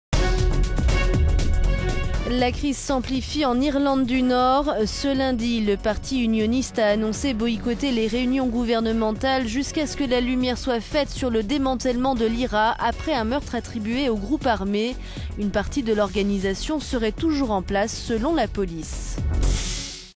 La crise s'amplifie en Irlande du Nord
Ce lundi, le parti unioniste démocrate (DUP) du Premier ministre a annoncé boycotter toutes les réunions gouvernementales jusqu'à que la lumière soit faite sur la réalité du démantèlement de l'IRA, après un meurtre attribué au groupuscule en août dernier. L'organisation disait avoir déposé les armes il y a dix ans, mais selon la police, une partie de son organisation serait "toujours largement en place".…